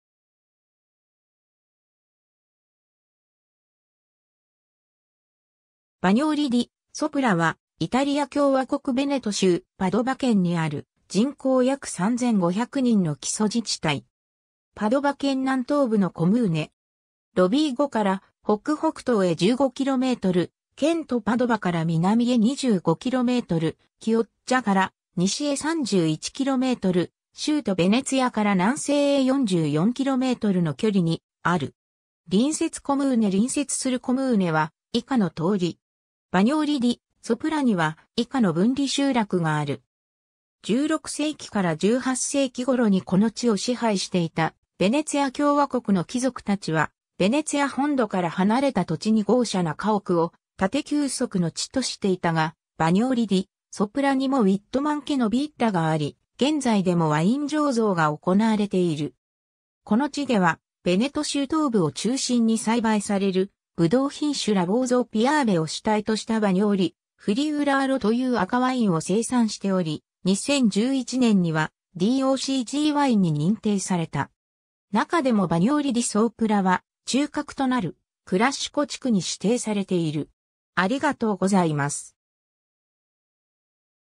バニョーリ・ディ・ソプラ
バニョーリ・ディ・ソプラ（イタリア語: Bagnoli di Sopra）は、イタリア共和国ヴェネト州パドヴァ県にある、人口約3,500人の基礎自治体（コムーネ）。
 パドヴァ県南東部のコムーネ。
ロヴィーゴから北北東へ15km、県都パドヴァから南へ25km、キオッジャから西へ31km、州都ヴェネツィアから南西へ44kmの距離にある。
 バニョーリ・ディ・ソプラには、以下の分離集落（フラツィオーネ）がある。
 16世紀から18世紀頃にこの地を支配していたヴェネツィア共和国の貴族達は、ヴェネツィア本土から離れた土地に豪奢な家屋（ヴィッラ Villa）を建て休息の地としていたが、バニョーリ・ディ・ソプラにもウィッドマン家のヴィッラがあり、現在でもワイン醸造が行われている。
 この地では、ヴェネト州東部を中心に栽培されるブドウ品種ラボーゾ・ピアーヴェ（別名フリウラーロ）を主体としたバニョーリ・フリウラーロ (Bagnoli Friularo) という赤ワインを生産しており、2011年にはD.O.C.G.ワインに認定された。
なかでもバニョーリ・ディ・ソープラは中核となるクラッシコ地区に指定されている。